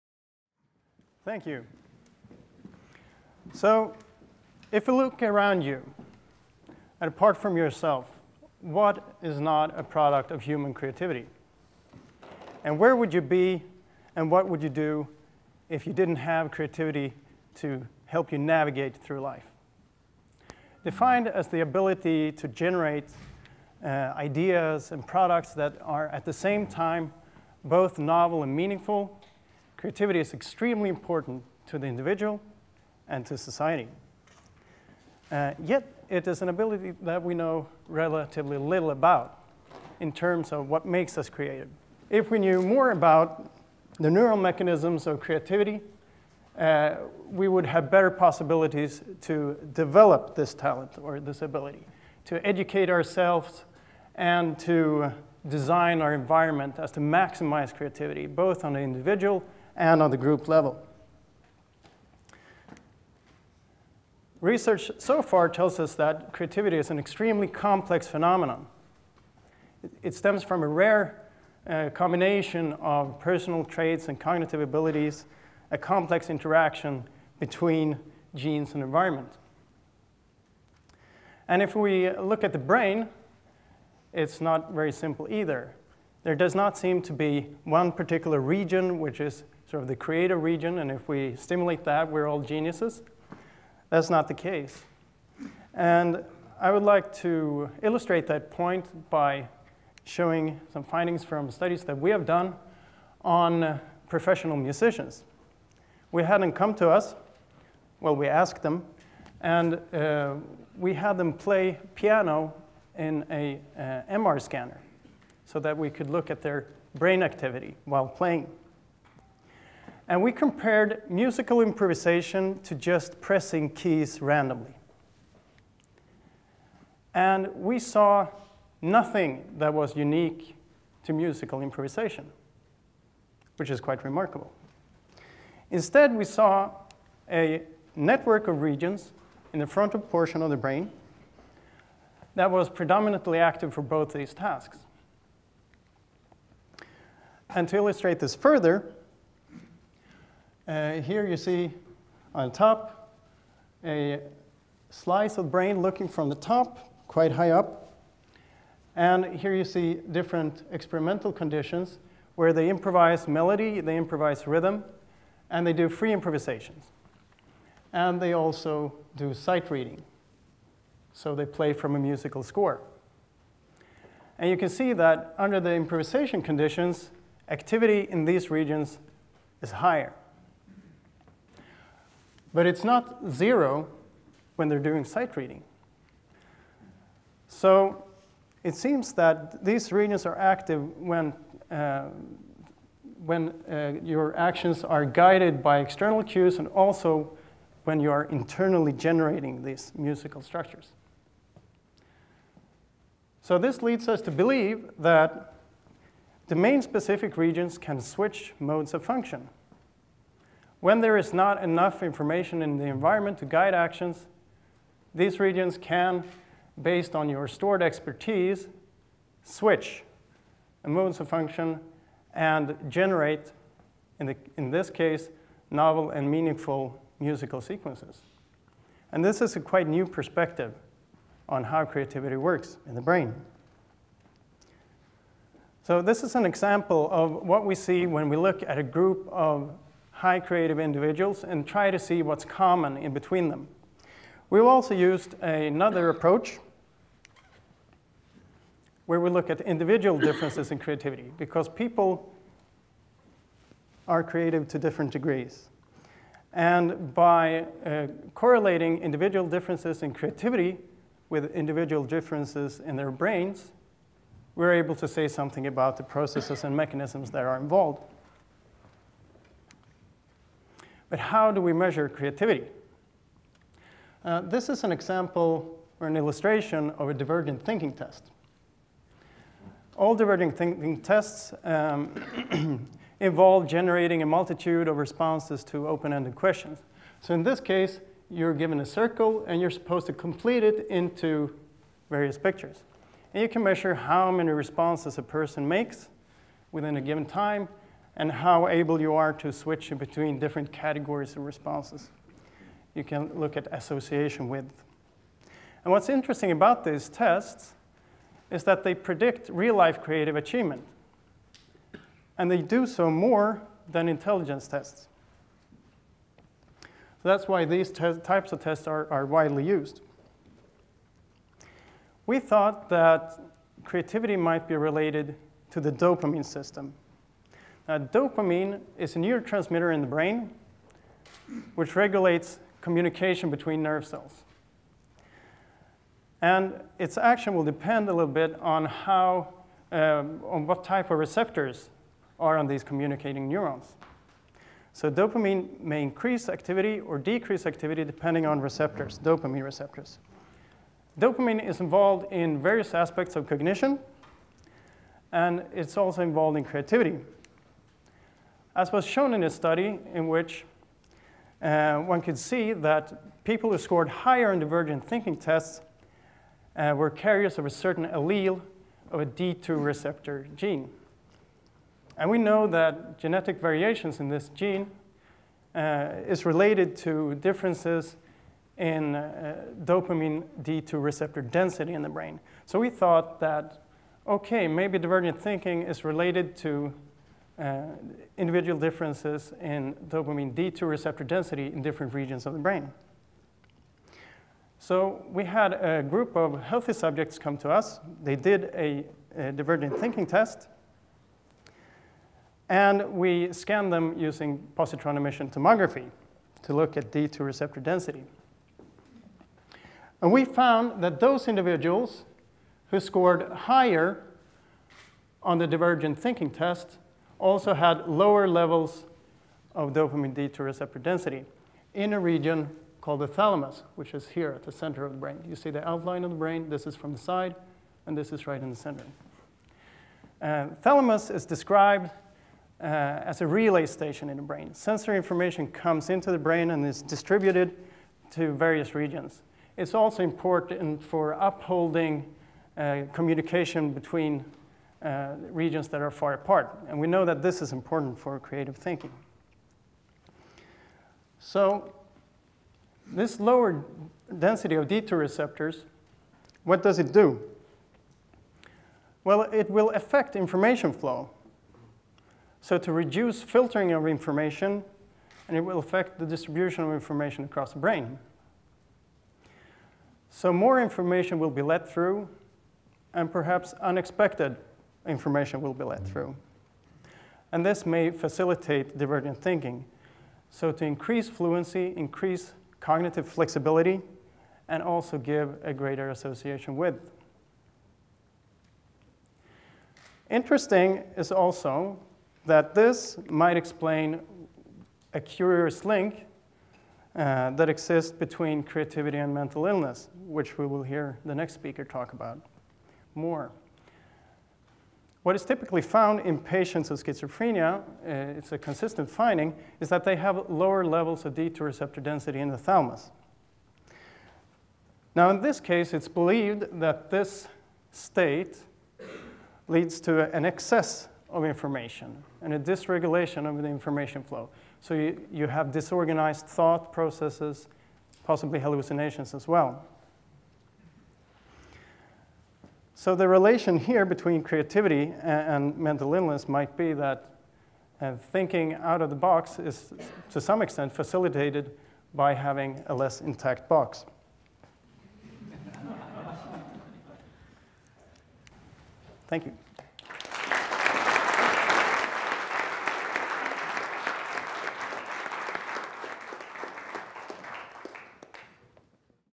TEDxKarolinskaInstitutet - Örjan de Manzano - Neural mechanisms in creativity
By highlighting the link between mental illness and creativity, Dr. Örjan de Manzano advocates that thinking outside of the box is to some extent facilitated by having a less intact box. de Manzano explains that highly creative people who do well on divergent tests have a lower density of dopamine D2 receptors in the thalamus than less creative people. Since schizophrenics are also known to have low D2 density in this part of the brain, de Manzano argues that this suggests a cause of the link between mental illness and creativity.

Örjan de Manzano is a researcher at Karolinska Institutet, Sweden. He recently presented his PhD thesis "Biological mechanisms in creativity and flow", in which he presents a series of investigations into the neural underpinnings of creative thinking and musical improvisation.